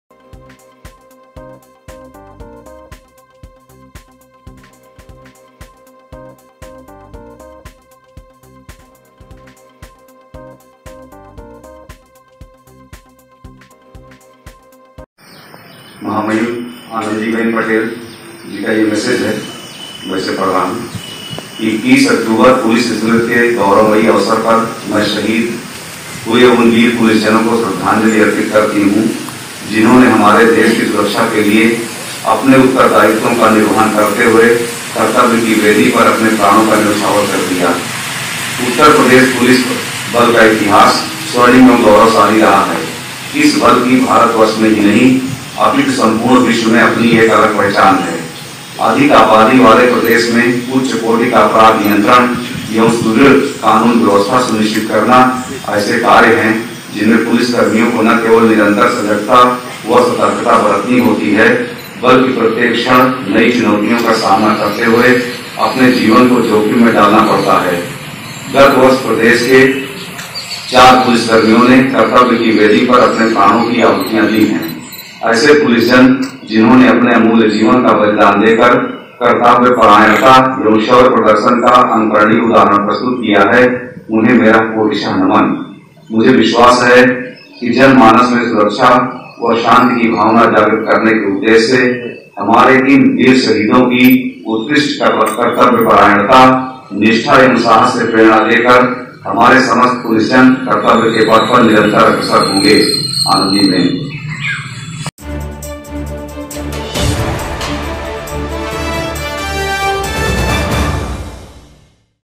पुलिस स्मृति दिवस पर आयोजित हुई श्रद्धांजलि सभा, परिजनों को मिला स्मृति चिन्ह
गाजीपुर पुलिस लाइन में “पुलिस स्मृति दिवस” के अवसर परंपरागत तरीके से श्रद्धांजलि सभा का आयोजन किया गया। पुलिस अधीक्षक रामबदन सिंह ने लाइन परिसर में स्थित स्मृति स्थल पर पुष्पांजलि कर ड्यूटी में तैनात शहीद जवानों को याद किया और उन्हें भावभीनी श्रद्धांजलि अर्पित की। एसपी गाज़ीपुर ने उपस्थित पुलिस अधिकारियों और कर्मियों को उत्तर प्रदेश की राज्यपाल आनंदीबेन पटेल, मुख्यमंत्री योगी आदित्यनाथ तथा डीजीपी हितेशचंद्र अवस्थी के संदेशों को पढ़कर सुनाया।